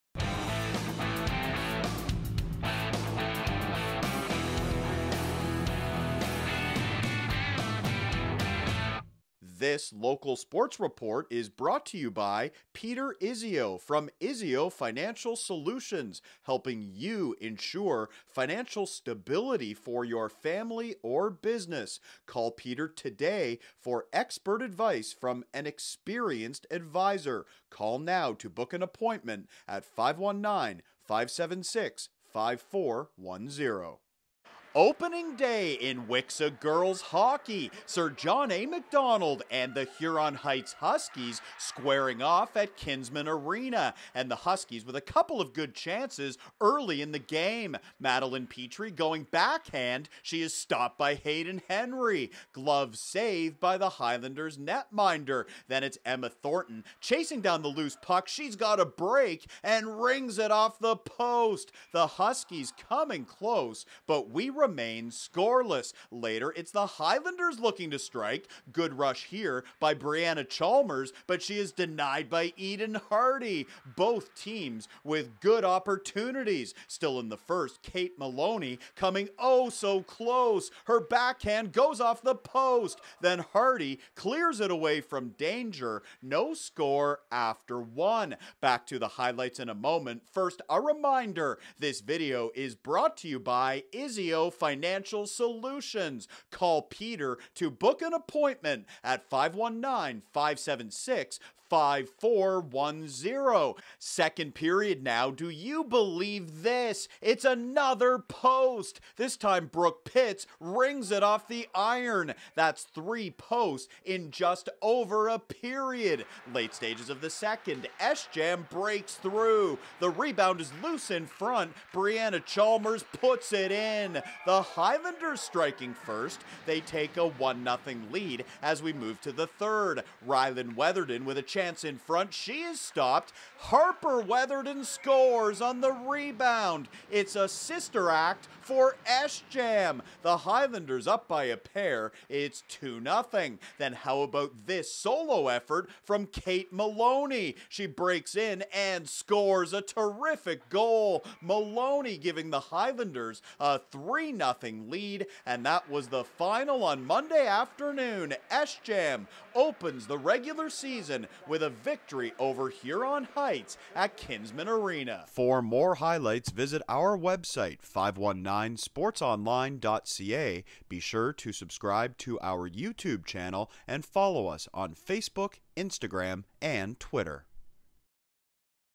WCSSAA Girls Hockey - Huron Heights vs Sir John A. Macdonald
Highlights from Monday December 2nd between the Huskies and Highlanders at Kinsmen Arena.